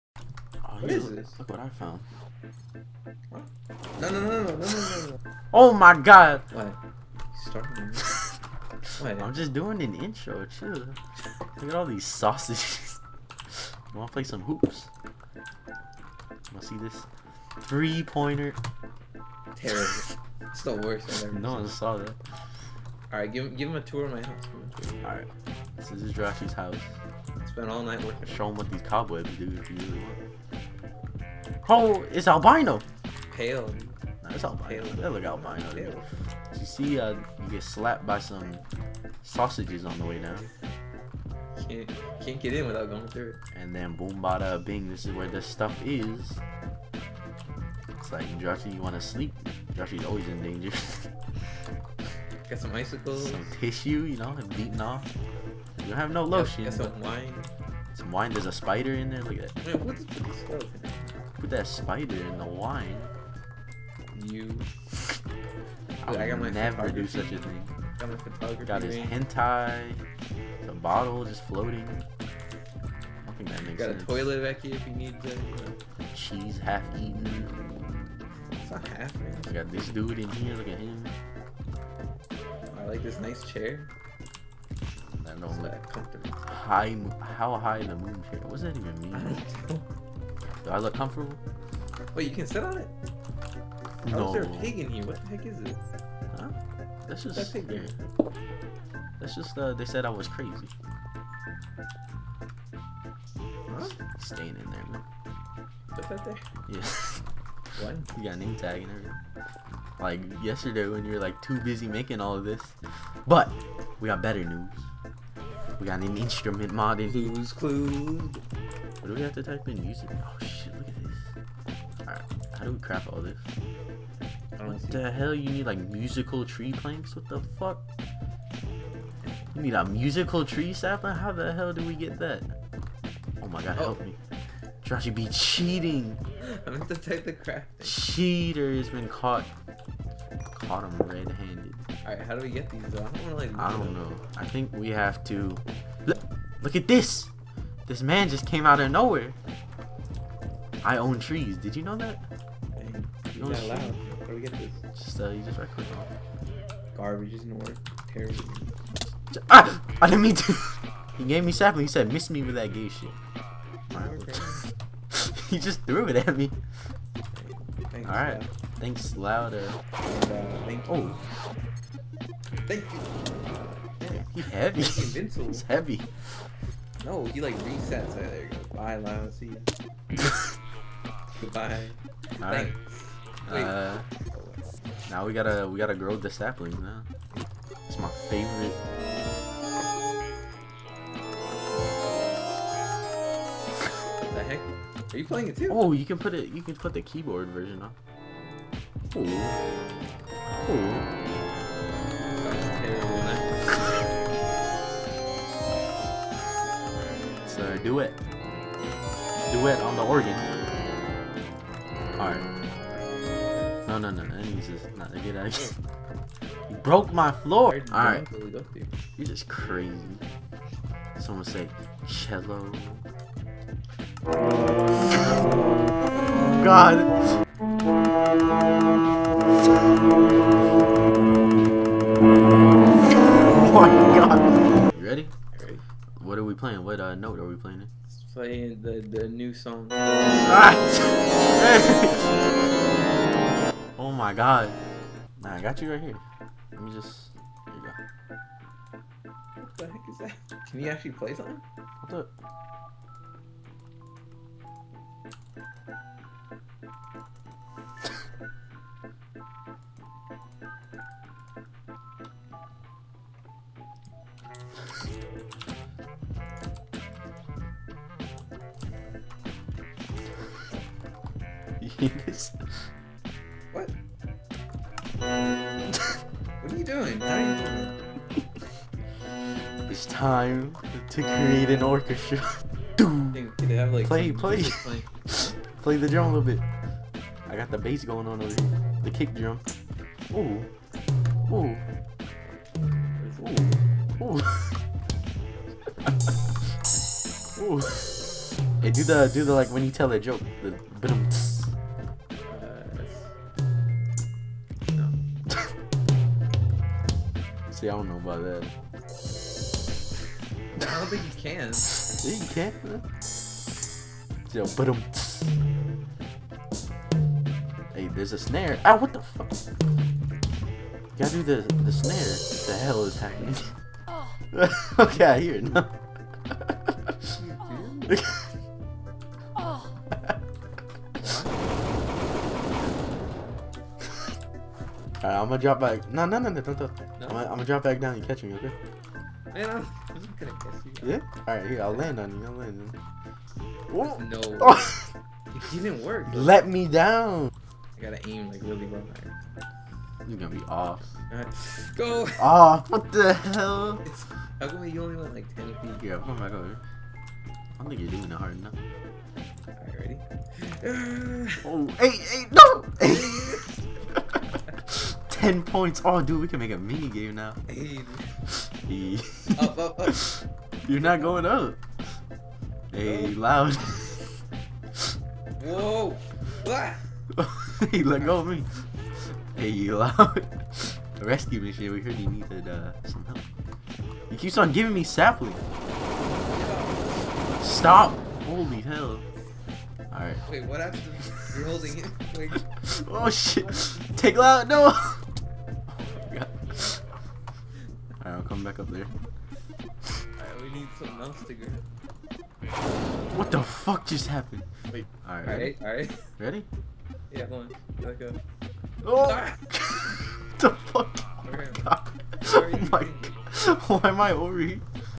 Revenge of the Gypsy Voodoo King Ep.4
We install the musicraft mod...Hell breaks loose.